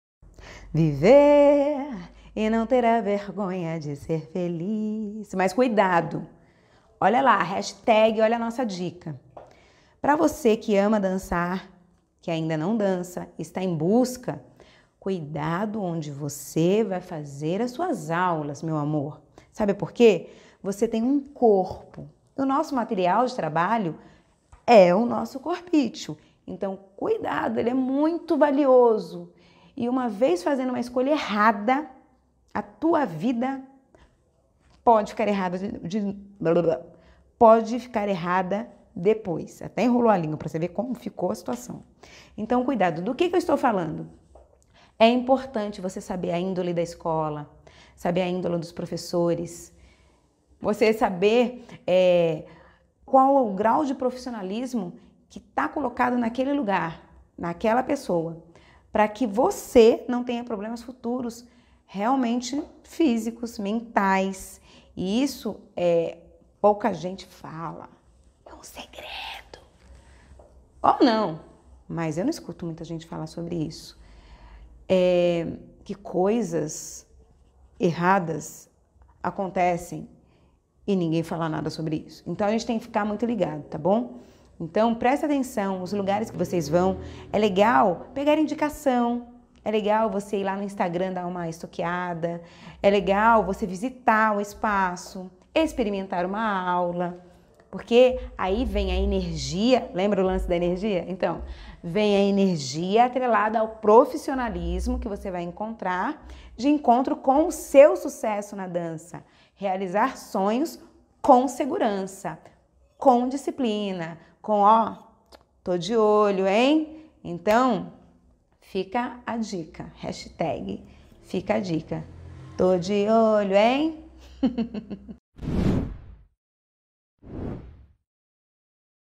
SeLigaNaDuo - Desvendando o mundo da dança de forma simples
Descobrimos que muita gente tem dúvidas com relação ao mundo dança, então resolvemos ajudar pq informação é tudo. Se você também gosta de estar bem informado e tem alguma dúvida sobre o nosso mundo da dança escreva nos comentários essa ação nos ajudará ajudar você e mais pessoas.